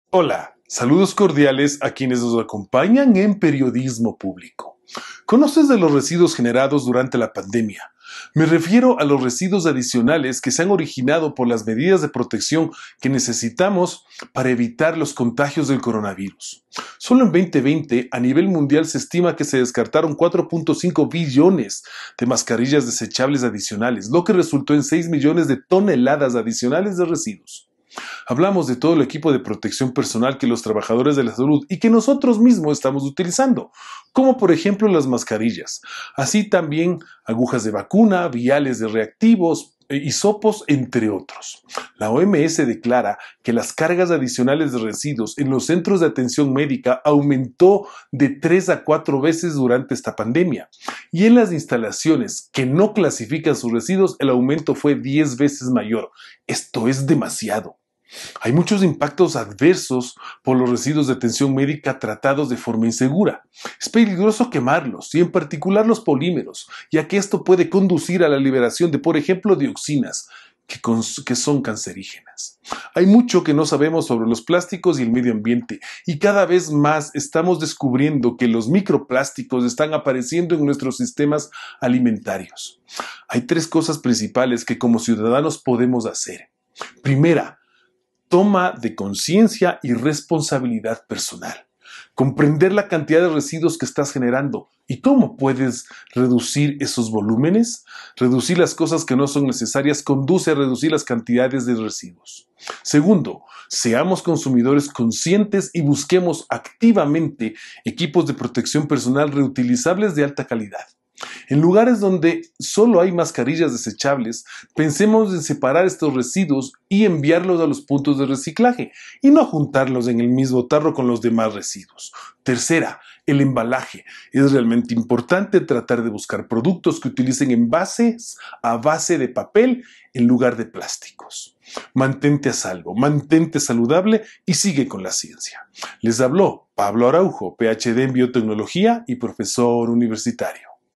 Atención con los residuos de la pandemia – 04.02.2022
¿Conoces de los residuos generados durante la pandemia? Me refiero a los residuos adicionales que se han originado por las medidas de protección que necesitamos para evitar los contagios del coronavirus.

Solo en 2020, a nivel mundial se estima que se descartaron 4,5 billones de mascarillas desechables adicionales, lo que resultó en 6 millones de toneladas adicionales de residuos.

Hablamos de todo el equipo de protección personal que los trabajadores de la salud y que nosotros mismo estamos utilizando, como las mascarillas, así como, de agujas de vacuna, viales, reactivos de prueba, hisopos, entre otros.

Editorial presentada en Periodismo Público Ecuador el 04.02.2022